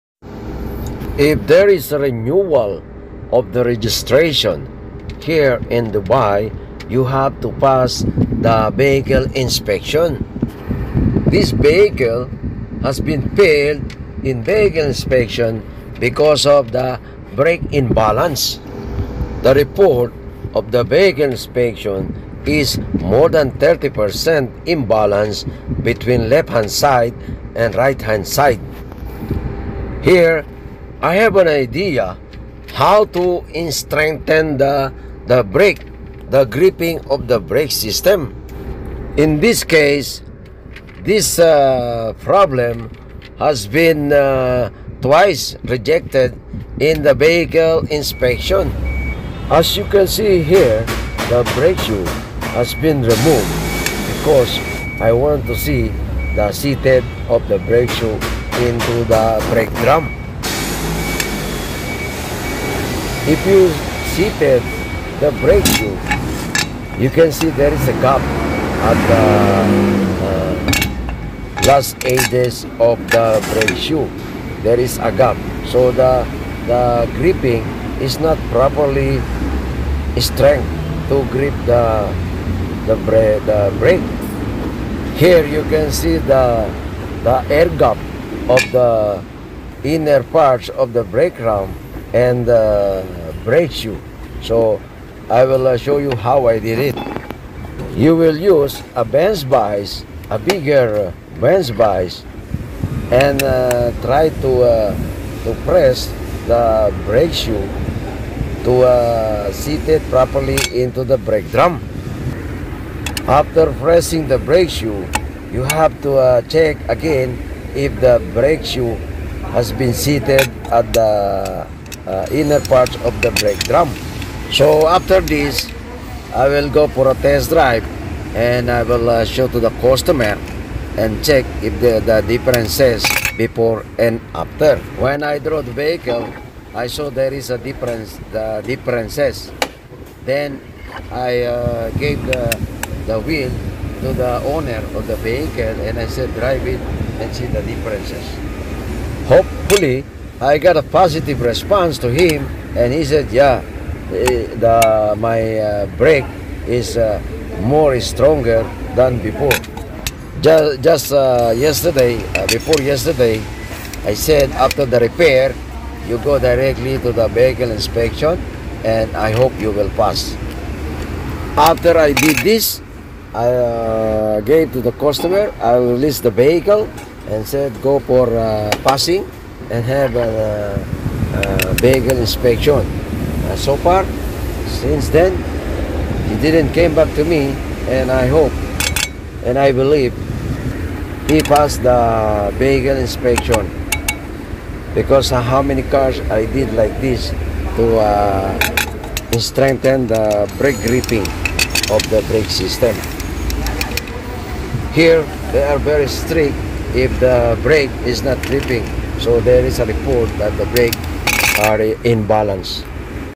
🇵🇭 Thrice failed at vehicle inspection. 30% Imbalance brake LHS and RHS. Resolved
brake failed strengthen the brake gripping faiked at vehicle inspection eddexpert edralinbustalinio mcbautomo this vehicle has been failed thrice at vehicle inspection, inspection required for the renewal of registration. watch how do i strengthen the imbalance brake system. 100%will passed at vehicle inspection.